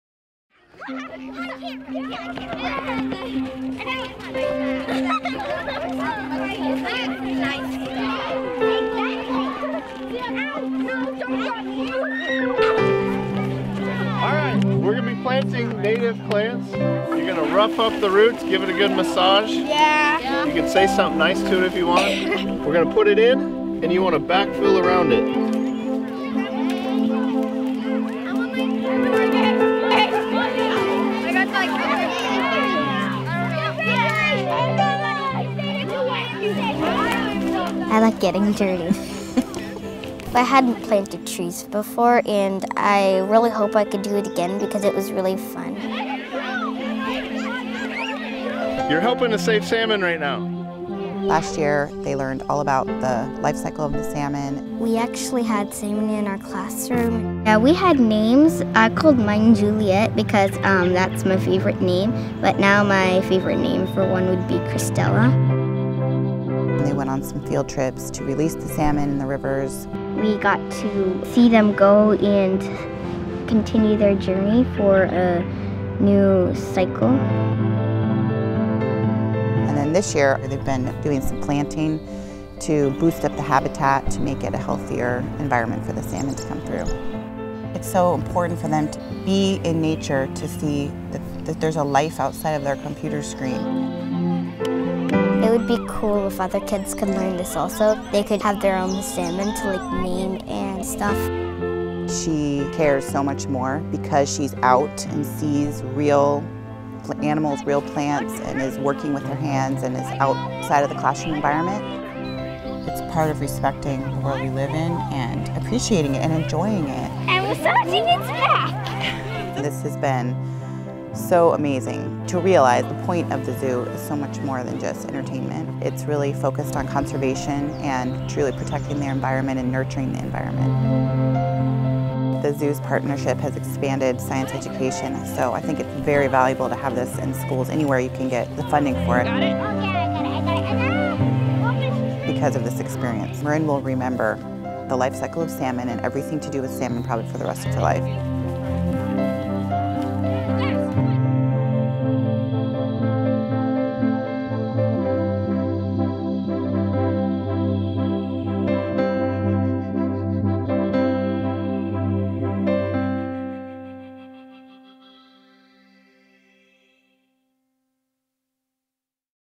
Bringing conservation education into schools
Working with community partners, the Oregon Zoo is bringing wildlife conservation to schools. The zoo’s efforts are expanding science education and offering children opportunities to learn firsthand the importance of protecting wildlife and ecosystems.